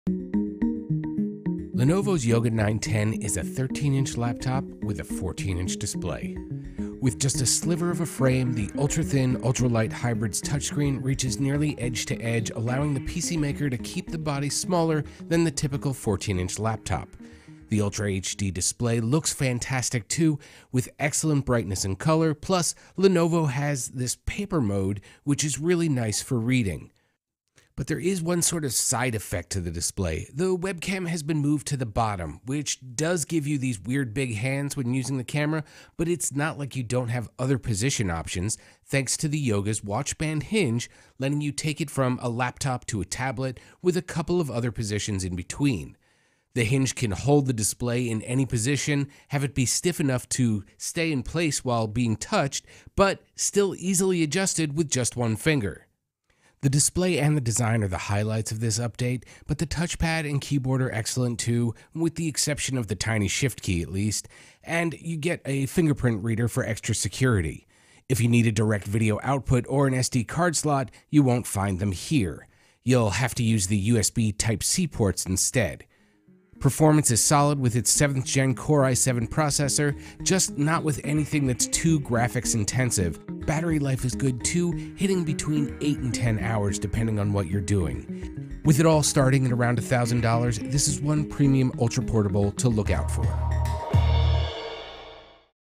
Lenovo says 'bye-bye bezel' for the premium hybrid Yoga 910
Its ultra HD display goes nearly edge-to-edge giving you more room to work and play for hours and hours.